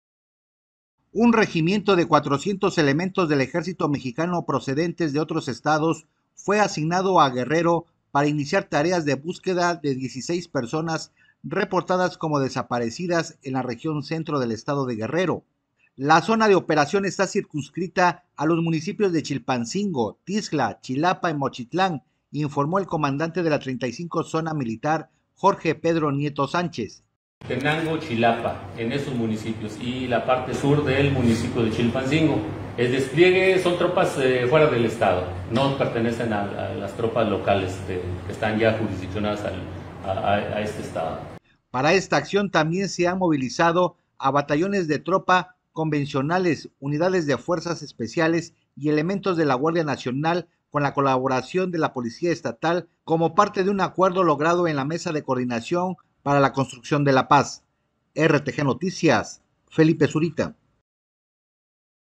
RTGnoticias | Ejercito asigna fuerza especial de búsqueda de los 17 desaparecidos
Un regimiento de 400 elementos del Ejército mexicano procedente de otros estados fue asignado a Guerrero para iniciar tareas de búsqueda de las 17 personas reportadas como desaparecidas en la región Centro del estado.